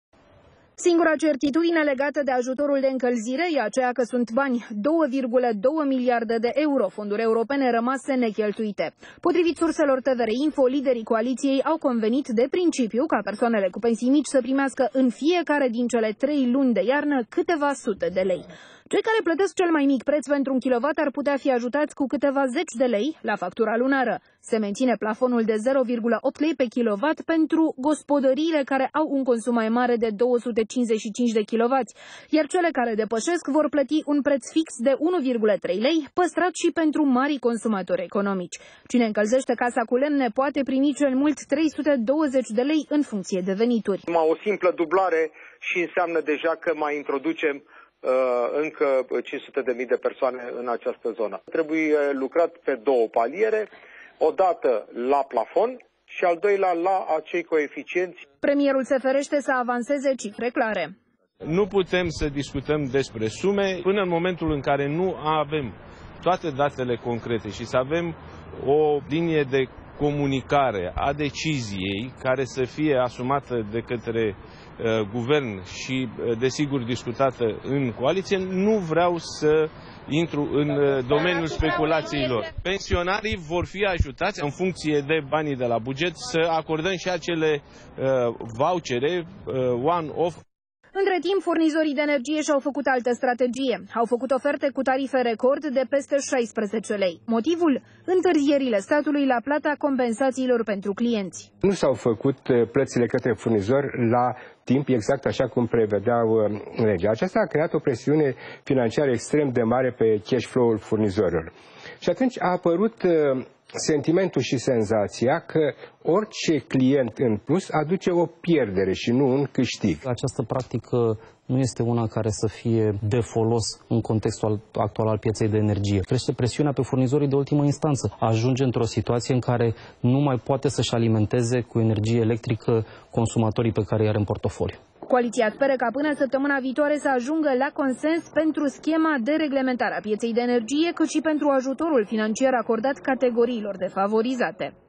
Preț maxim pentru un kilowatt și ajutoare în bani
Coaliția caută soluții de susținere financiară a populației și a firmelor afectate de prețurile mari din energie. Între timp, ofertele unora dintre furnizori au ajuns și la 16 lei pe kilowatt. Liderii coaliției iau în calcul să extindă actualele forme de ajutor pentru un segment mai mare de populație și să cheltuiască peste două miliarde de euro, bani europeni, pentru vouchere de încălzire și energie electrică, dar și alți bani, în special pentru pensionarii cu venituri mici.